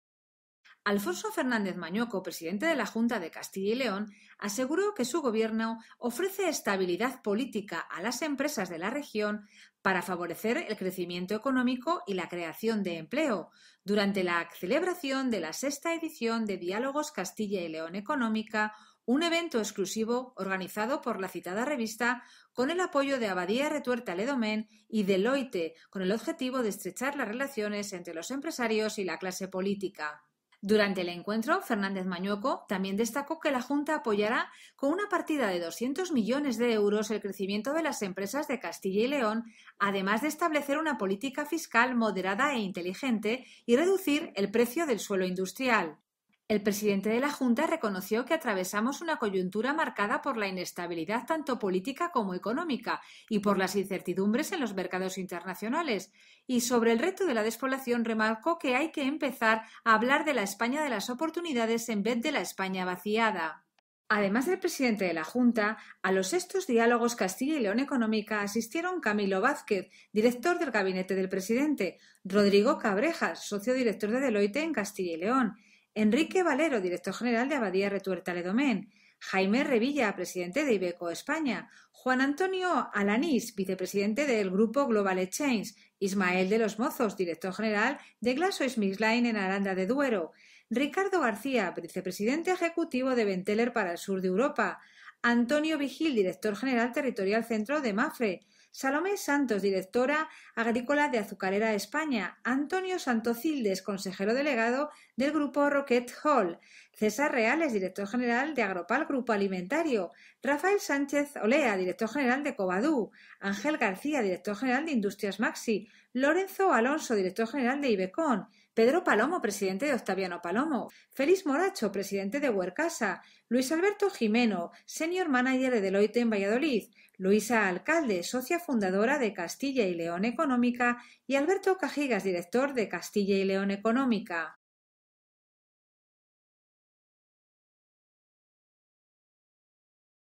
VI Diálogos Castilla y León Económica
Alfonso Fernández Mañueco, presidente de la Junta de Castilla y León, aseguró que su Gobierno “ofrece estabilidad política” a las empresas de la región para favorecer el crecimiento económico y la creación de empleo, durante la celebración de la VI edición de Diálogos Castilla y León Económica, un evento exclusivo organizado por la citada revista con el apoyo de Abadía Retuerta LeDomaine y Deloitte con el objetivo de estrechar las relaciones entre los empresarios y la clase política.